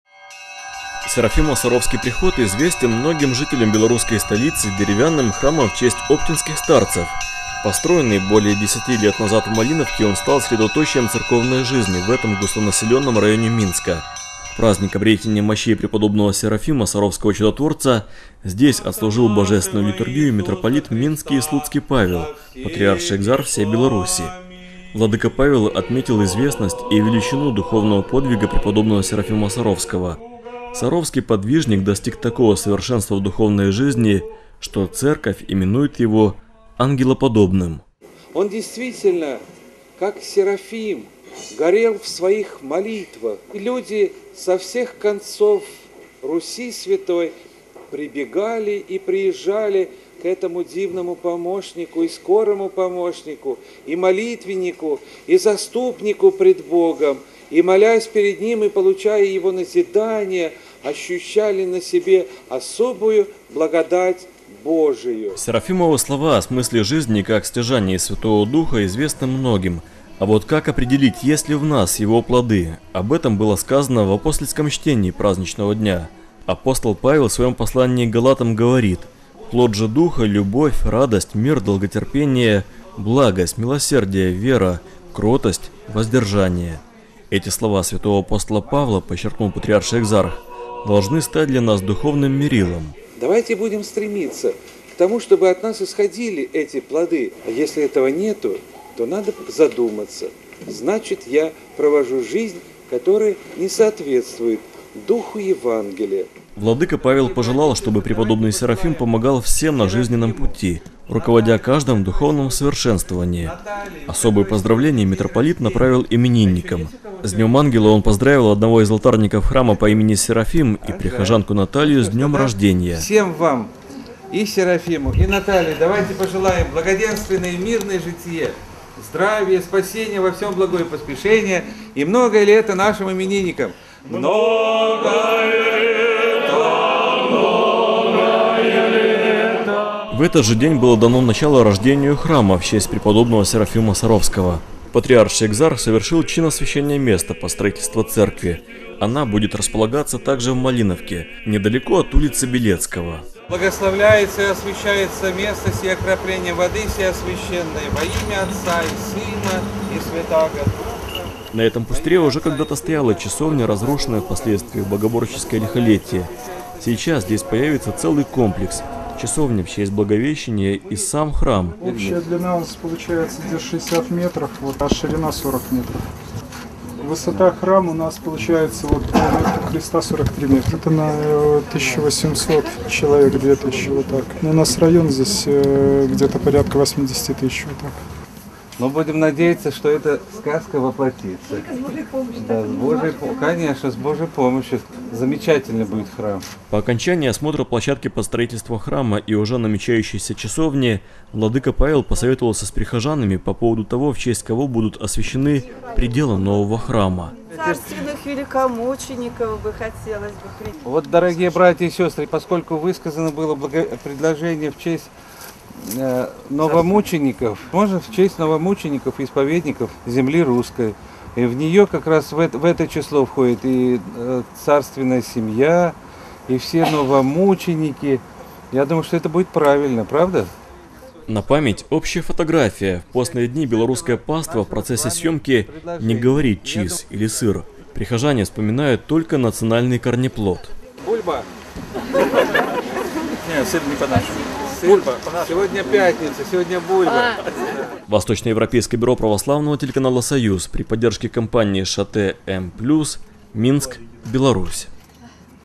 В Минске будет построен храм в честь преподобного Серафима Саровского
В день памяти обретения мощей преподобного Серафима, Саровского чудотворца, в белорусской столице состоялось знаменательное событие. Патриарший Экзарх всея Беларуси, Митрополит Минский и Слуцкий Павел, освятил место под строительство храма в честь преподобного Серафима с приделом в честь новомучеников и исповедников земли Русской.